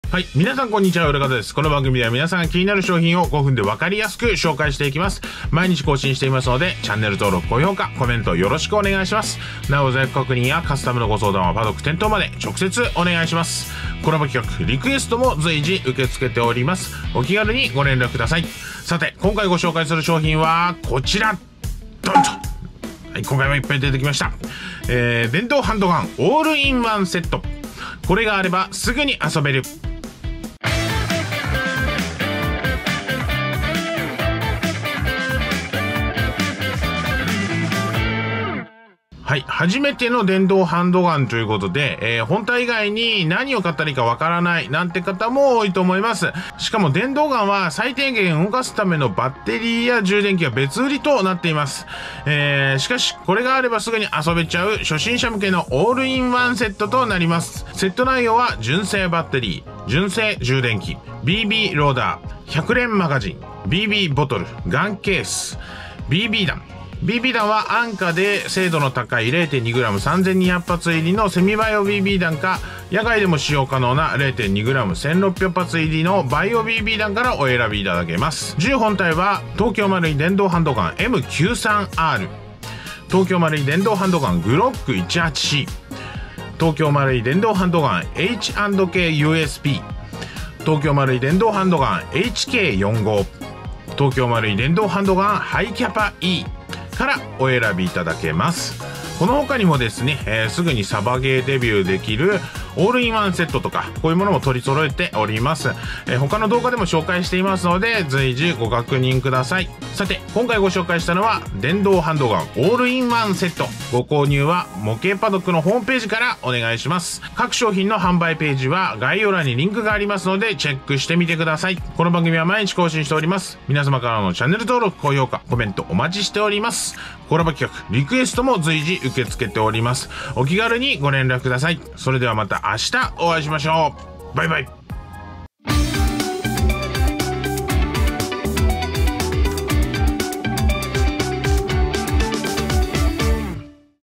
【5分でわかる】東京マルイ 電動ハンドガン オールインワンセットのご紹介【Vol.157】モケイパドック サバゲー 初心者 サバゲーデビュー！ エアガンセット
この番組では皆さんが気になる商品を、5分で分かりやすく紹介していきます！なお、在庫確認やカスタムのご相談はパドック店頭まで直接お願いします！

今回ご紹介する商品は...
初めての電動ガン！本体以外に何を買ったら良いか分からない！？なんて方もいますよね？電動ガンは最低限動かす為のバッテリーや充電器は別売りとなっています！しかし！これが有ればサバゲーデビュー出来ちゃう！初心者向けのオールインワンセットとなります！

セット内容は…
●純正バッテリー
●純正充電器
●BBローダー
●100連マガジン
●BBボトル
●ガンケース
●BB弾
安価で精度の高い0.2g3200発入りのセミバイオBB弾か、屋外でも使用可能な0.2g1600発入りのバイオBB弾からお選びください！

銃本体は…
●東京マルイ 電動ハンドガン M93R
セット価格：22,204円(税込)

●東京マルイ 電動ハンドガン GLOCK 18C
セット価格：20,167円(税込)

●東京マルイ 電動ハンドガン H&K USP
セット価格：21,185円(税込)

●東京マルイ 電動ハンドガン HK45
セット価格：23,222円(税込)

●東京マルイ 電動ハンドガン Hi-CAPA E

...から選ぶことが可能です！

この他にも、直ぐに遊べるスターティングセットや電動ガンのお買い上げ時にのみ購入できる・モバイルバッグ＆急速充電器5種フルセットなども取り揃えています！他の動画でも随時ご紹介しますのでご確認ください！

Presenter：URAKATA
Cameraman：KUMAKICHI XL / S.NAMBA
Editor：URAKATA / KUMAKICHI XL / S.NAMBA
Filming Location： PADDOCK Shooting Range / STUDIO ISKYent.